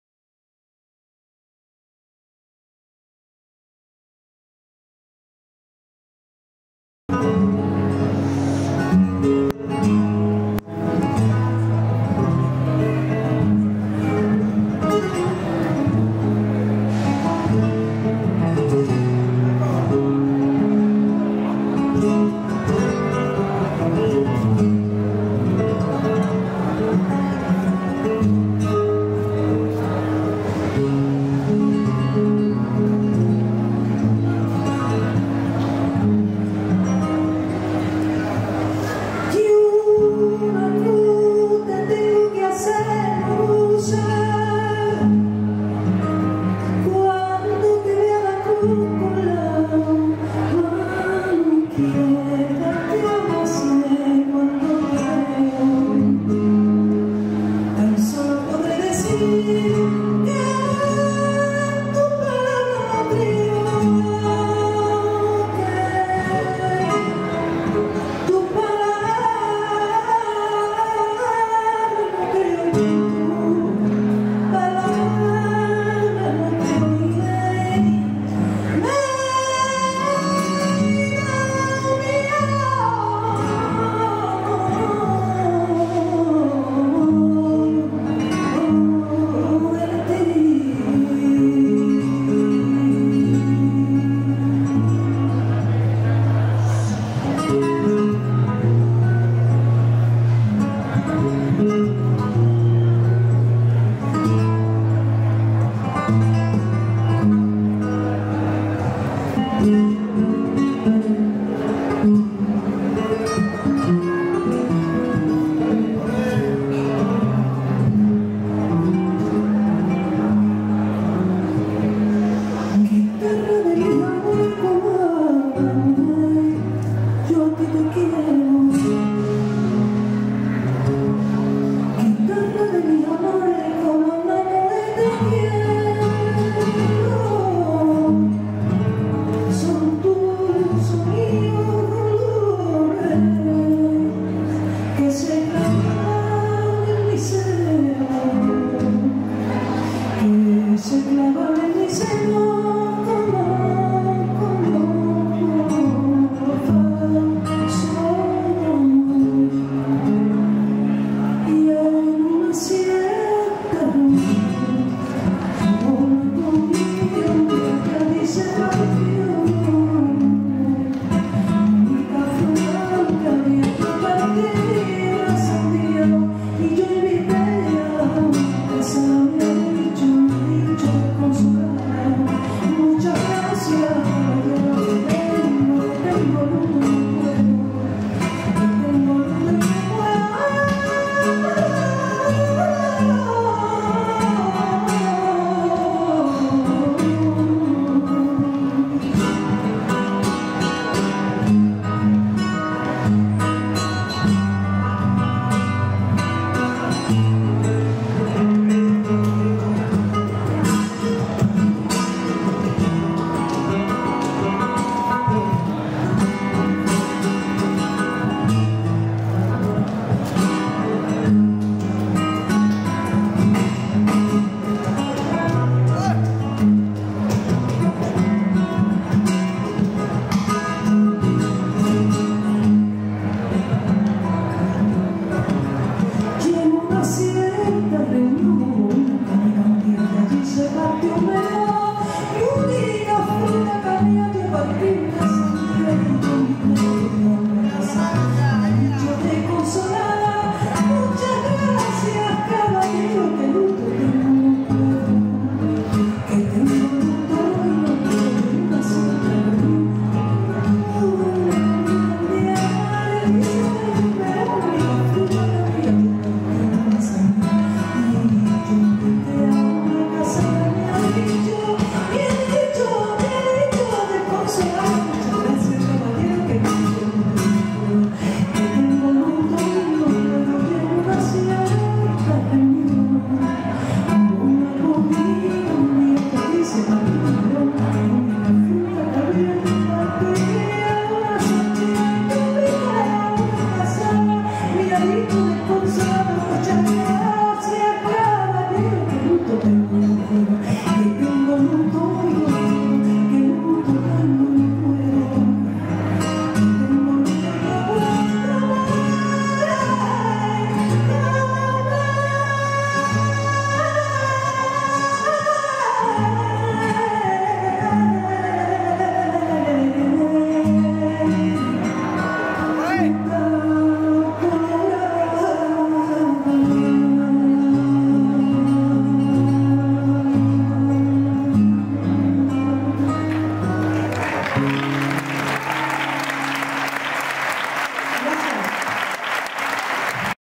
Sandra Carrasco, XXXII Noche Flamenca Pedro el de la Timotea, Herrera, Sevilla
Sandra Carrasco, Paco Cruz
XXXII Noche Flamenca Pedro el de la Timotea, Herrera, Sevilla